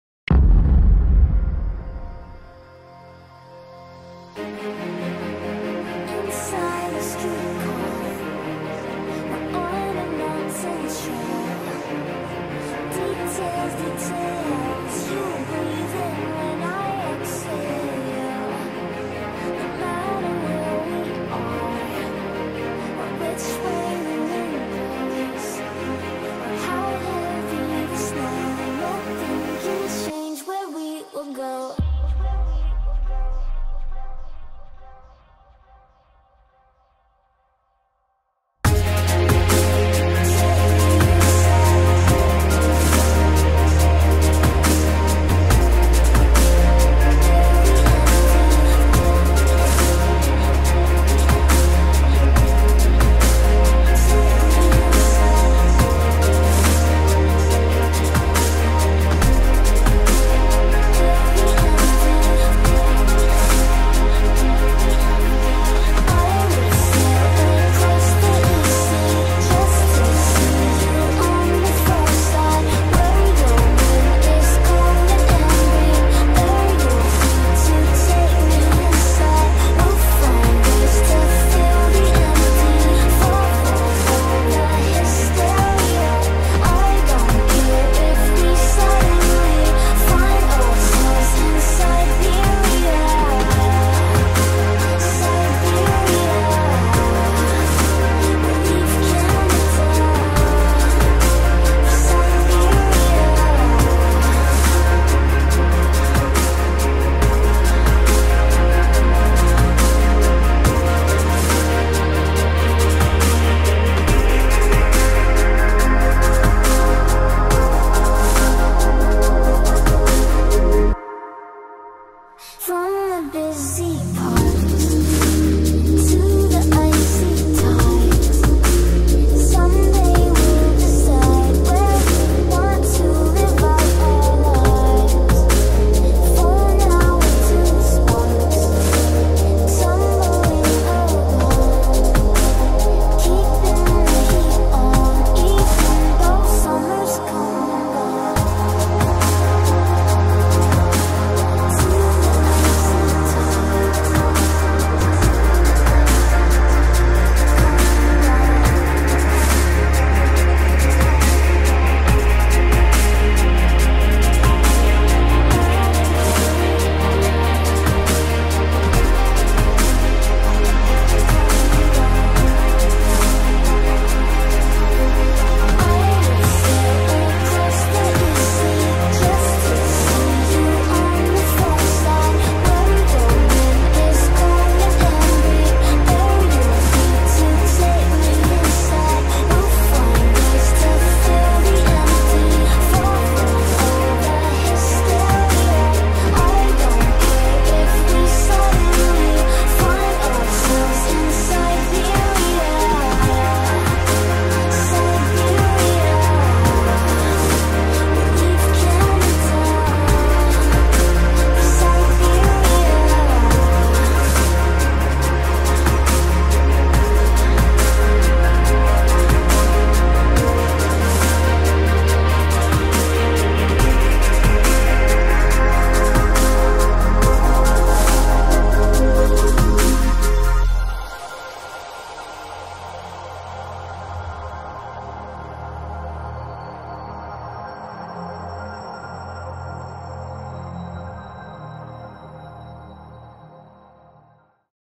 Epic hang gliding (Flying high in the snow mountains) Shot entirely with GoPro
A flight from Nova Gorica to snow mountains. Music from artist 5vel,